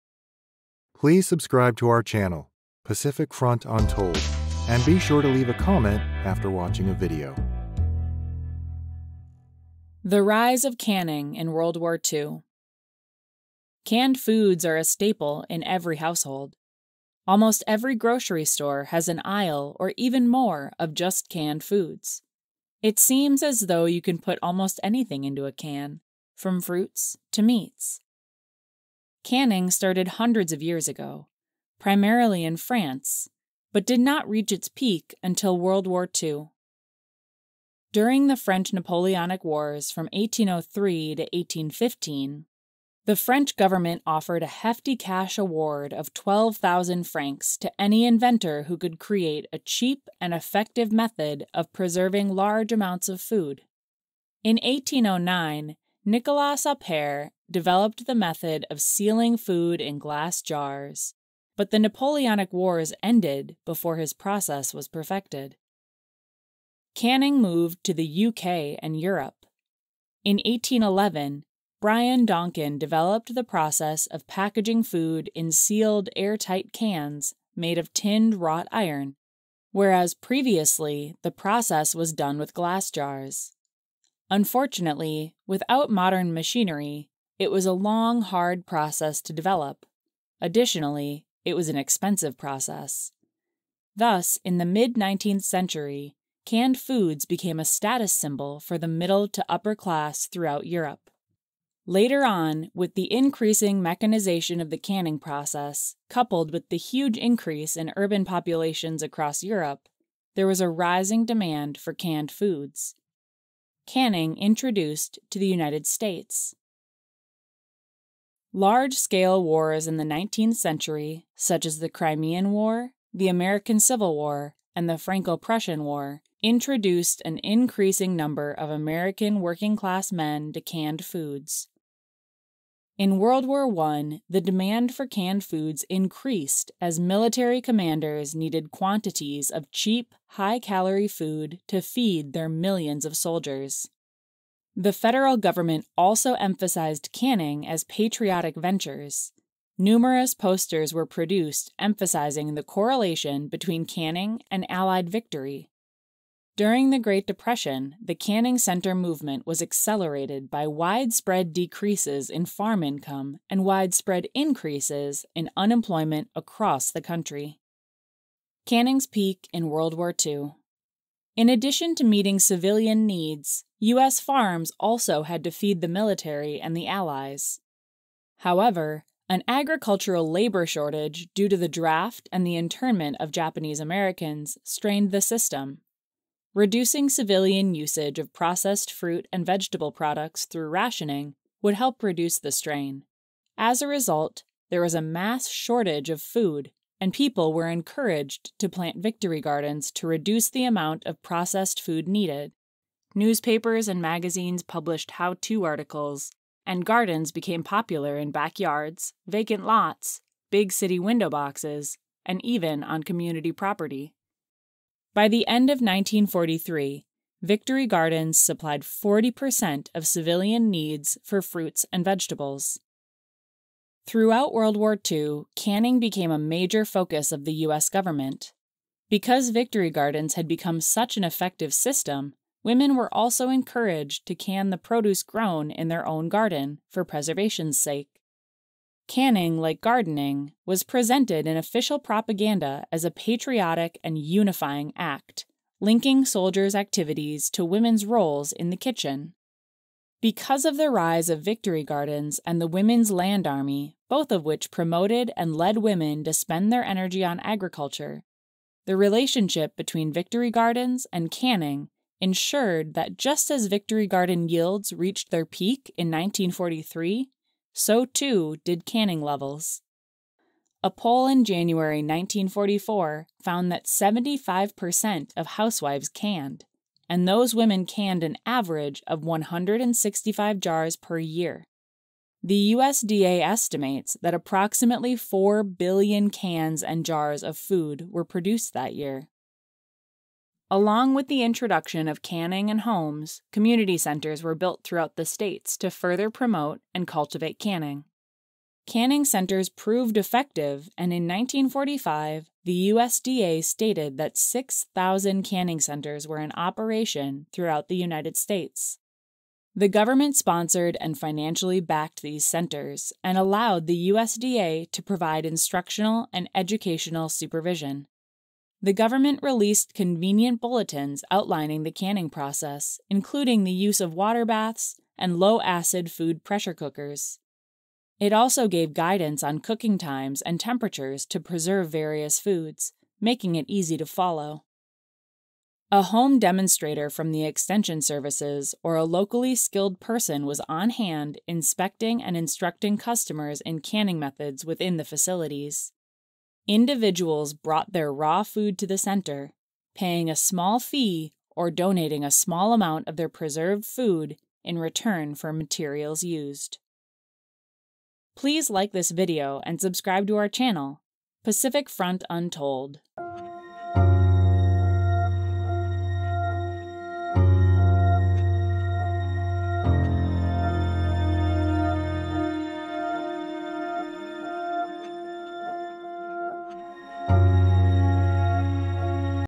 The Rise of Canning in WW2
Explore the fascinating journey of canning and its surge during World War II in our latest video! Starting in France during the Napoleonic Wars, canning evolved over centuries, with significant developments in the UK and Europe. It became popular in the United States during the 19th-century wars and World War I, ultimately peaking in World War II. Faced with food shortages and rationing, the US population turned to home canning and Victory Gardens, resulting in an estimated 4 billion cans and jars of food produced in 1943 alone. We'll delve into the role of community canning centers, the rise of home canning, and how it tied into the more considerable war effort. Join us on this journey through history, understanding the significant role of canning in society and war times.